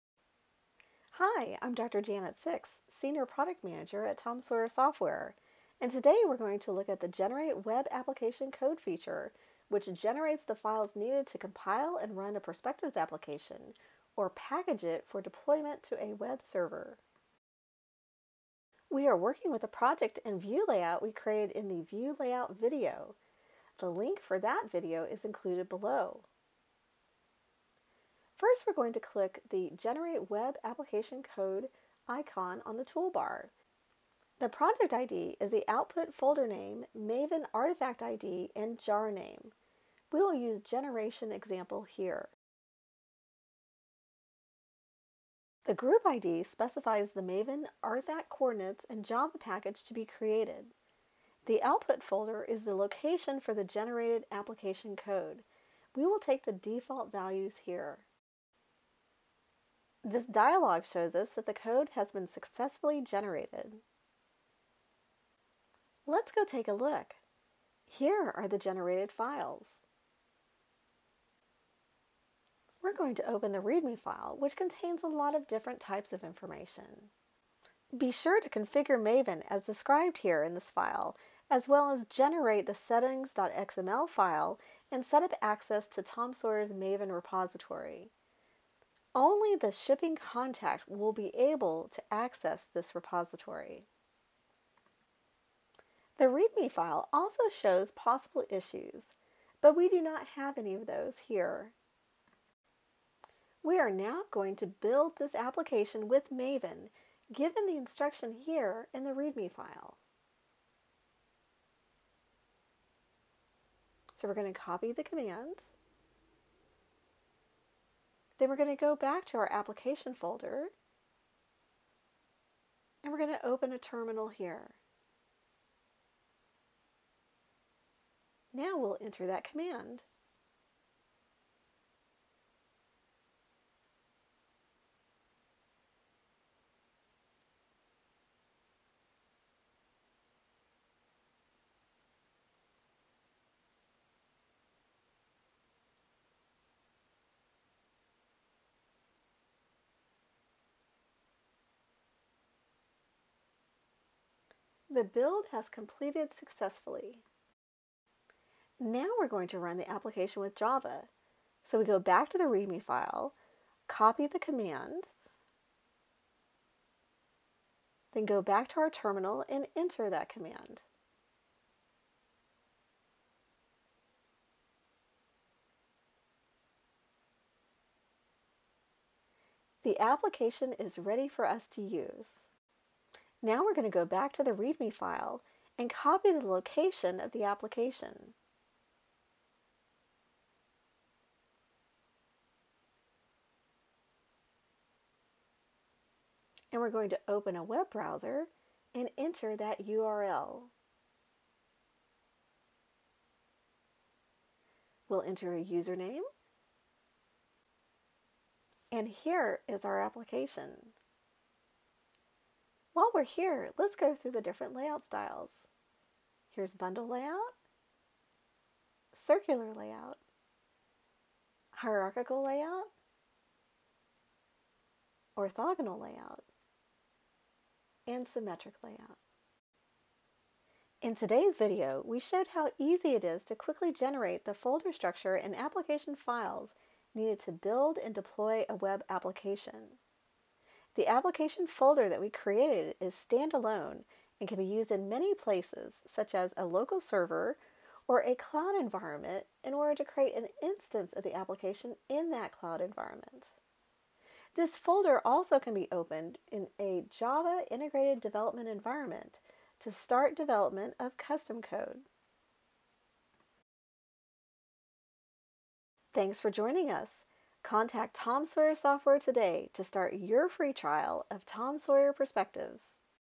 How to Generate Web Application Code
In this video, we show the Generate Web Application Code feature in Perspectives. This feature generates the files needed to compile and run the application or package it for deployment to a web server.  You can use this feature after using the View Layout feature to create a custom, dashboard-style arrangement of the views in your application.